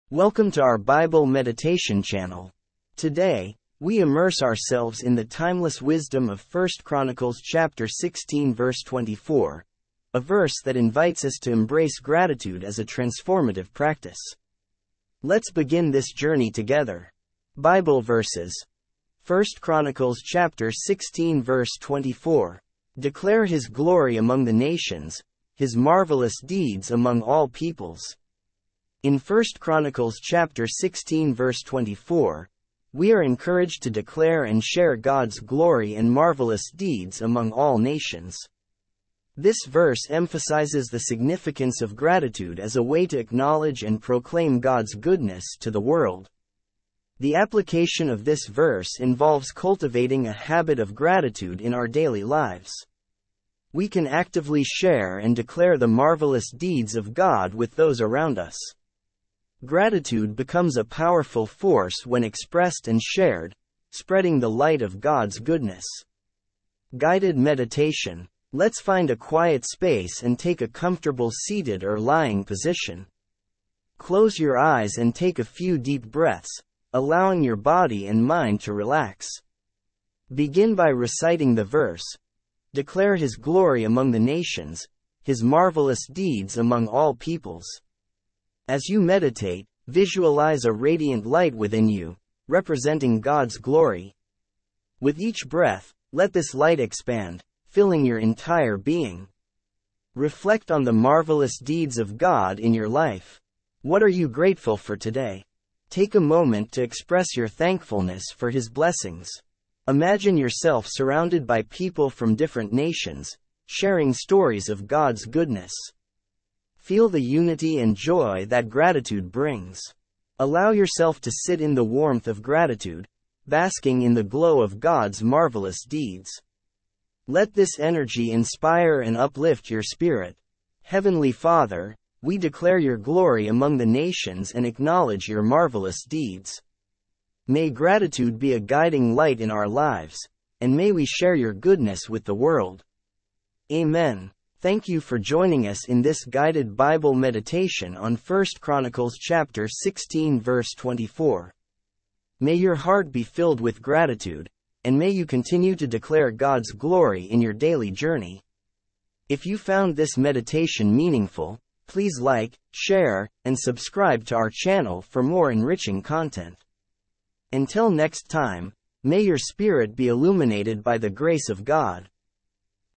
Embracing Gratitude: Meditating on 1 Chronicles 16:24 🙏
Join us in a soul-enriching Guided Bible Meditation as we delve into the profound wisdom of 1 Chronicles 16:24. Explore the essence of gratitude and its transformative power in daily life. 🌅🙏